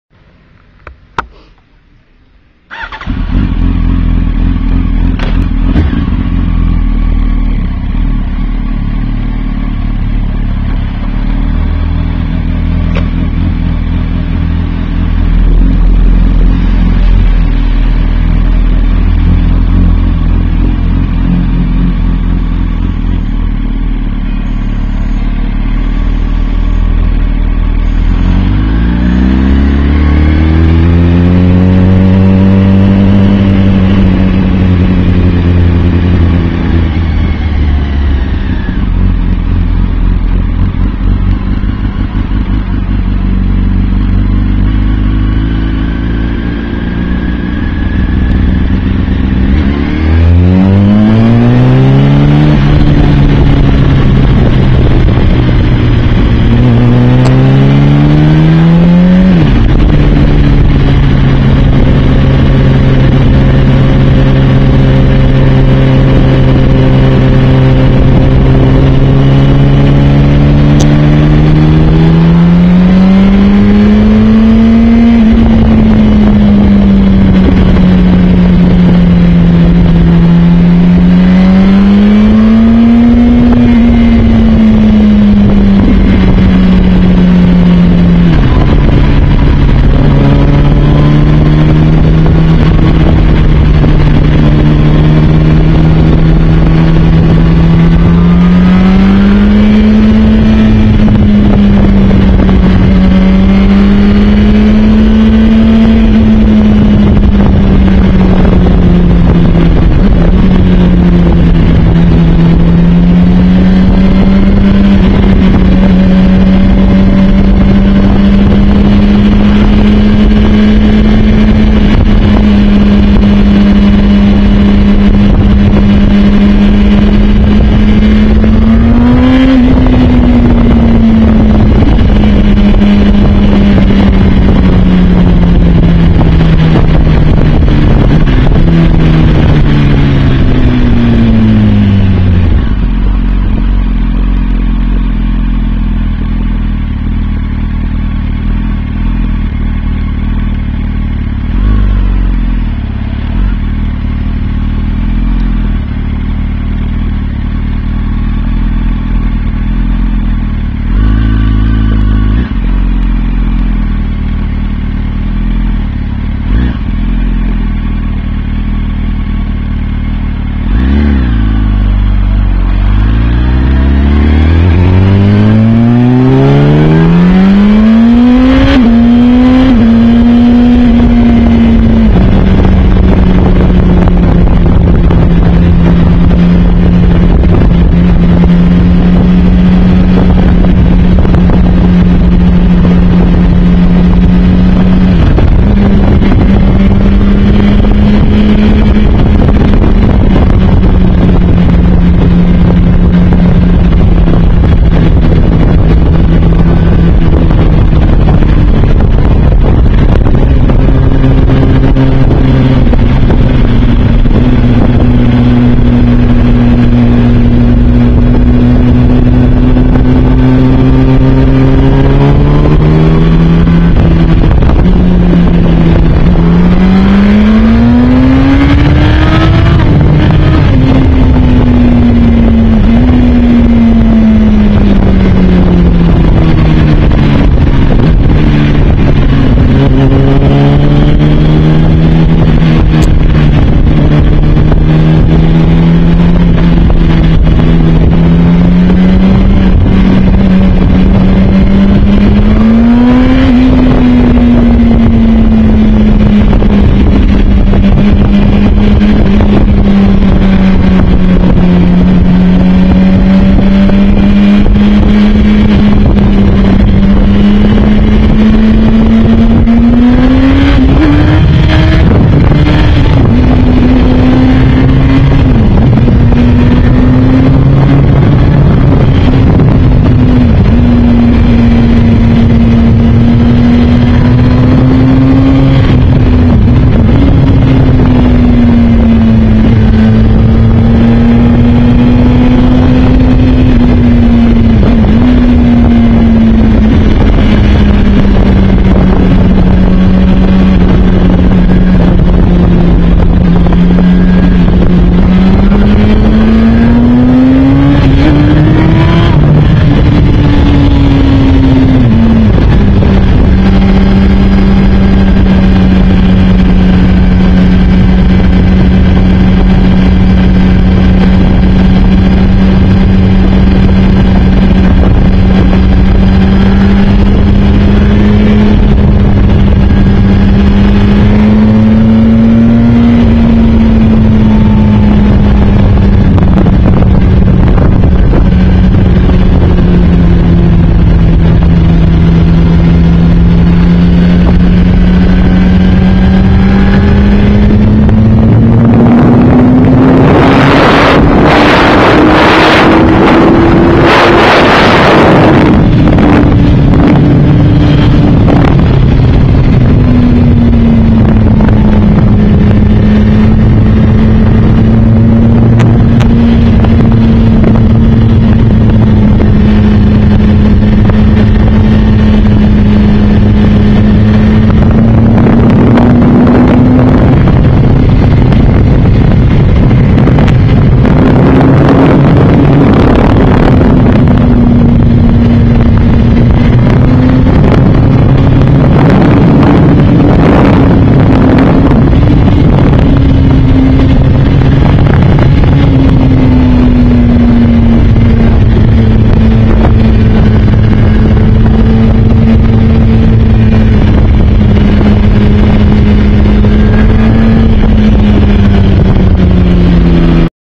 first ride in march 2010 with my R1 RN19(4)
nothing special. just a few km in this year 2 get a feeling 4 the bike